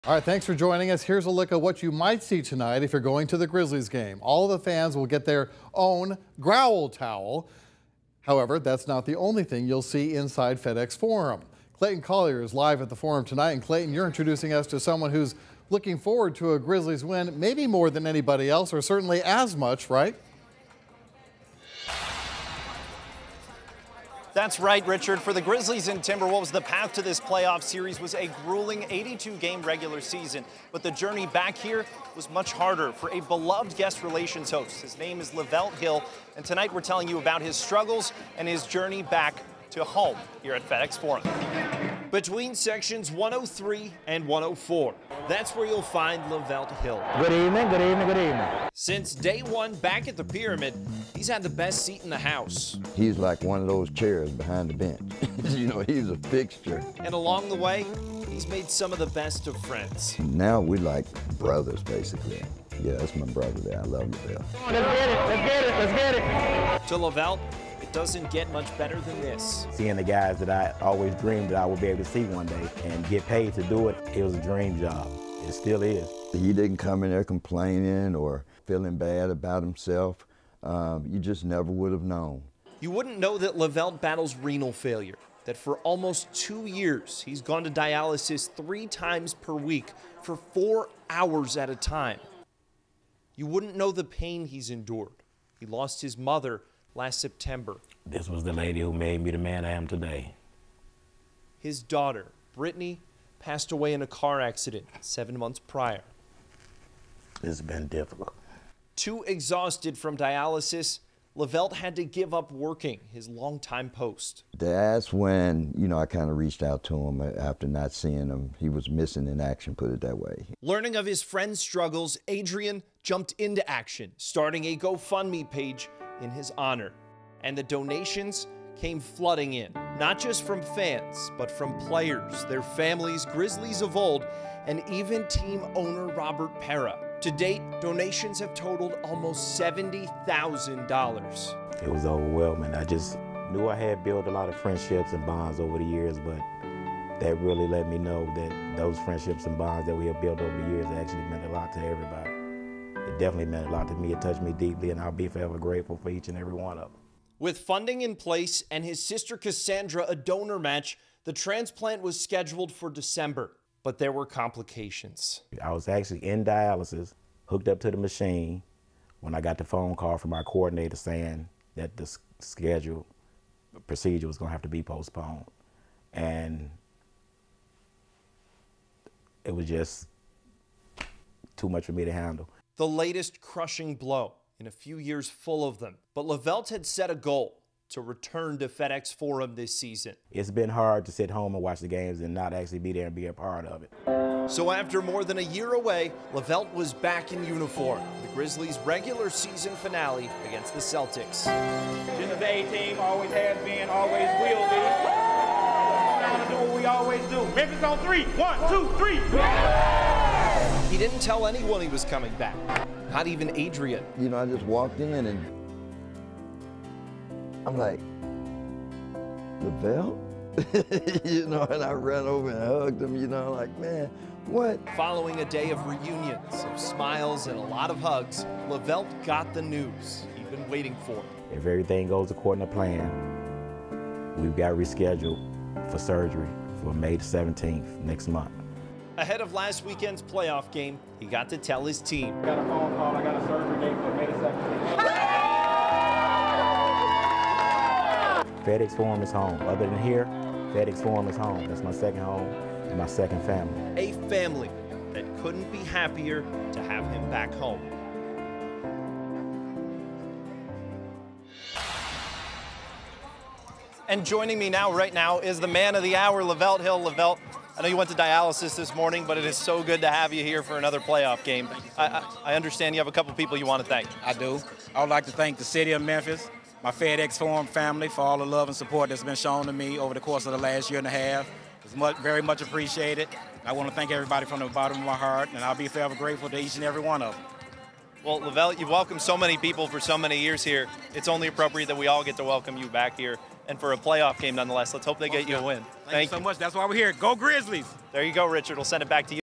The story of a longtime Grizzlies usher's journey back to the court from health problems
Memphis Grizzlies usher Lavelt Hill has been battling painful health problems for years. Just a few weeks ago, he made his way back to the FedEx Forum.